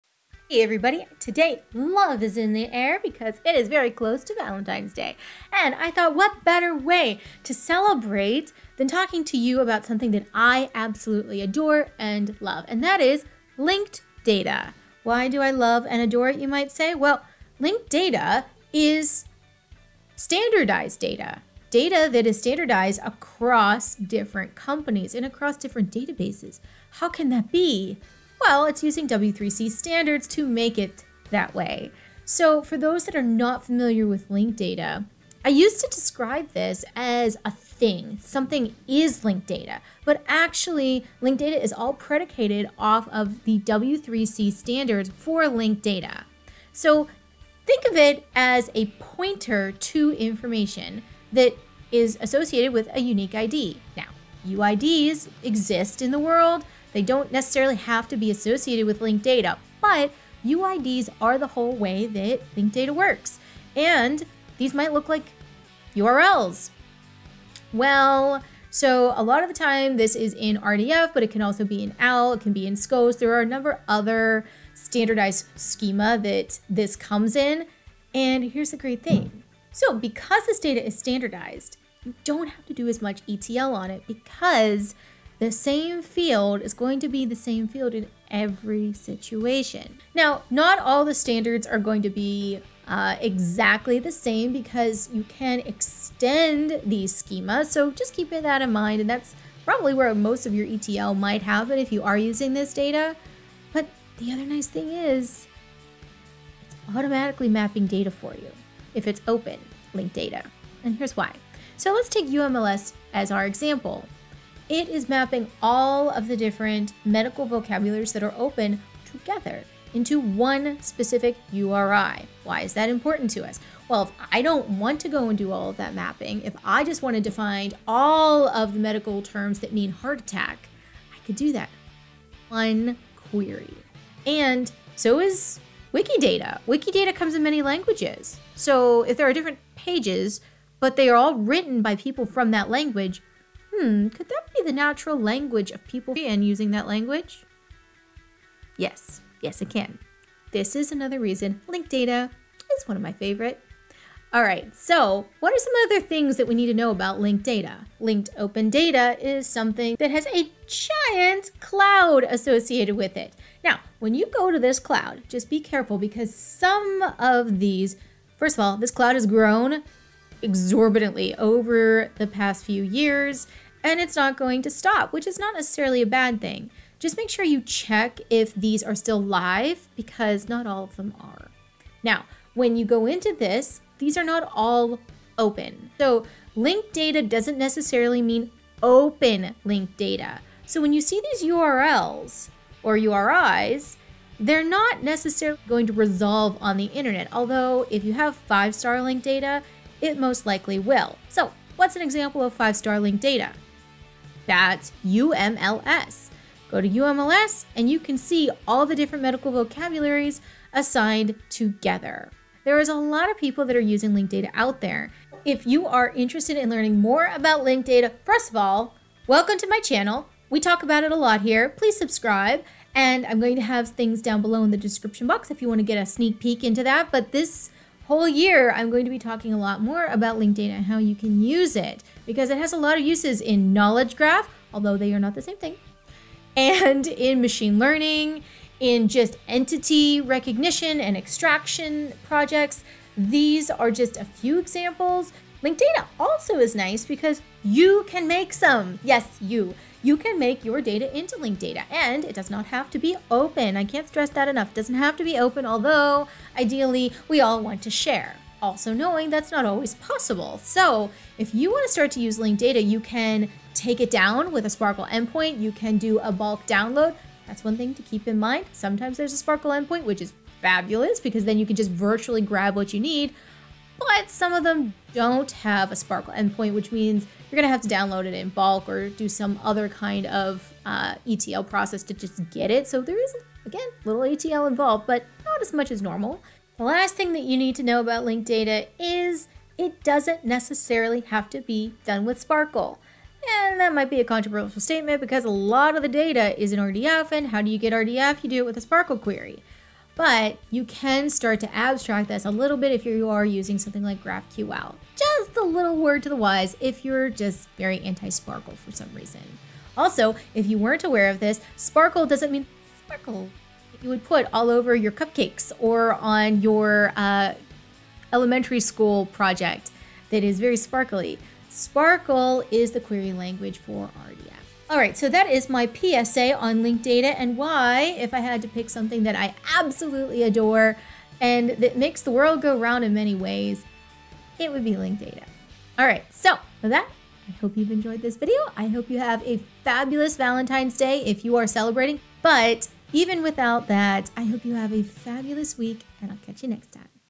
Why I LOVE 💖 Linked Data- And Maybe You Should Too
Linked Data has a lot of benefits, mapping between entities, sharing of data across dispersed databases, and making ETL way easier if you are using the base standards from W3C. Come check out some of the other things I love about Linked Data, and why you might want to go and check it out for yourself.